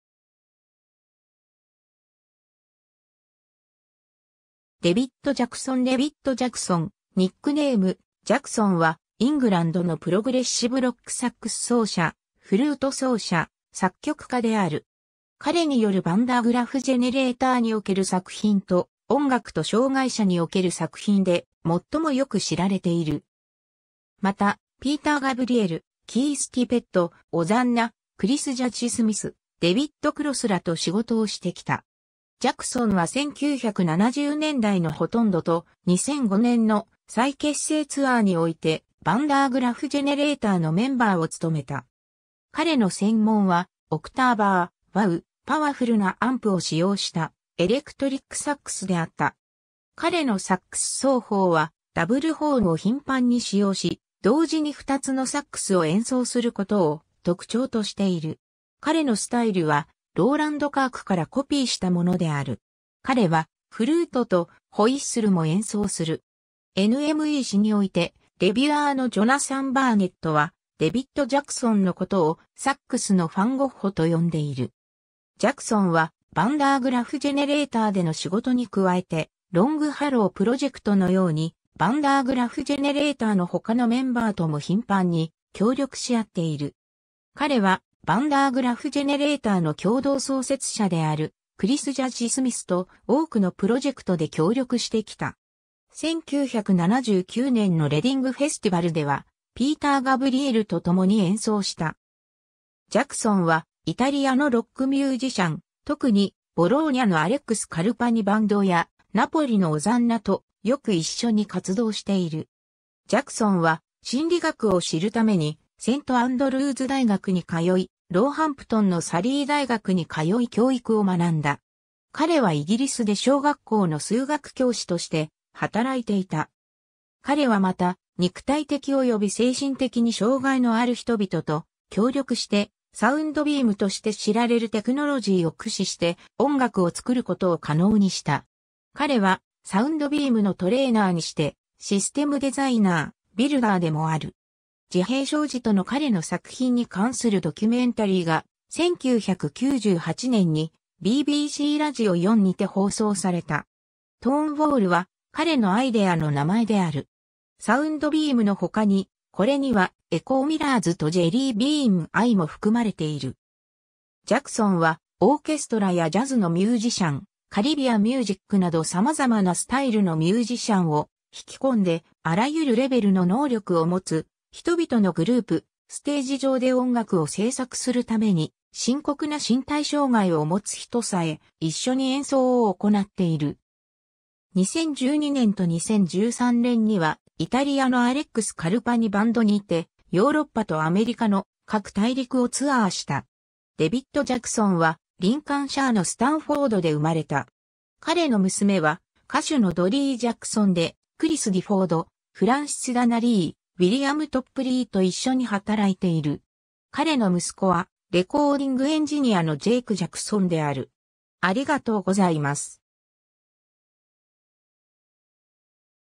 デヴィッド・ジャクソン
セント・アンドルーズ大学出身の人物
デヴィッド・ジャクソン デヴィッド・ジャクソン（David Jackson、1947年4月15日 - ）、ニックネーム : ジャクソン (Jaxon)は、イングランドのプログレッシブ・ロック・サックス奏者、フルート奏者、作曲家である。
彼によるヴァン・ダー・グラフ・ジェネレーターにおける作品と、音楽と障害者における作品で最もよく知られている。
また、ピーター・ガブリエル、キース・ティペット、オザンナ、クリス・ジャッジ・スミス、デヴィッド・クロスらと仕事をしてきた。
 ジャクソンは1970年代のほとんどと、2005年の再結成ツアーにおいてヴァン・ダー・グラフ・ジェネレーターのメンバーを務めた。
彼の専門は、オクターバー、ワウ、パワフルなアンプを使用したエレクトリック・サックスであった。
 彼のサックス奏法は、ダブルホーンを頻繁に使用し、同時に2つのサックスを演奏することを特徴としている。
彼のスタイルはローランド・カーク（ジャクソンに影響を与えたスタイルとテクニック）からコピーしたものである。
彼はフルートとホイッスルも演奏する。
『NME』誌において、レビュアーのジョナサン・バーネットは、デヴィッド・ジャクソンのことを「サックスのファン・ゴッホ (反逆者にして印象派であり、彼の個人的な収容所の窓から外の歪んだ世界についてのヴィジョンを分かち合っている)」と呼んでいる。
 ジャクソンはヴァン・ダー・グラフ・ジェネレーターでの仕事に加えて、ロング・ハロー・プロジェクトのように、ヴァン・ダー・グラフ・ジェネレーターの他のメンバーとも頻繁に協力し合っている。
彼は、ヴァン・ダー・グラフ・ジェネレーターの共同創設者であるクリス・ジャッジ・スミスと、多くのプロジェクトで協力してきた。
1979年のレディング・フェスティバルでは、ピーター・ガブリエルと共に演奏した。
ジャクソンはイタリアのロック・ミュージシャン、特にボローニャのアレックス・カルパニ・バンドや、ナポリのオザンナとよく一緒に活動している。
 ジャクソンは心理学を知るためにセント・アンドルーズ大学に通い、ローハンプトンのサリー大学に通い教育を学んだ。
彼はイギリスで小学校の数学教師として働いていた。
 彼はまた、肉体的および精神的に障害のある人々と協力して、「サウンドビーム (Soundbeam)」として知られるテクノロジーを駆使して音楽を作ることを可能にした。
彼は、サウンドビームのトレーナーにして、システム・デザイナー、ビルダーでもある。
自閉症児との彼の作品に関するドキュメンタリーが、1998年にBBCラジオ4にて放送された。
 「トーンウォール (Tonewall)」は彼のアイデアの名前である。
サウンドビームの他に、これにはエコーミラーズとジェリービーン・アイも含まれている。
ジャクソンは、オーケストラやジャズのミュージシャン、カリビアン・ミュージックなどさまざまなスタイルのミュージシャンを引き込んで、あらゆるレベルの能力を持つ人々のグループ、ステージ上で音楽を制作するために深刻な身体障害をもつ人さえ、一緒に演奏を行っている。
 2012年と2013年には、イタリアのアレックス・カルパニ・バンドにて、ヨーロッパとアメリカの各大陸をツアーした。
 デヴィッド・ジャクソンは、リンカンシャーのスタンフォードで生まれた。
彼の娘は歌手のドリー・ジャクソンで、クリス・ディフォード、フランシス・ダナリー、ウィリアム・トップリーと一緒に働いている。
彼の息子はレコーディング・エンジニアのジェイク・ジャクソンである。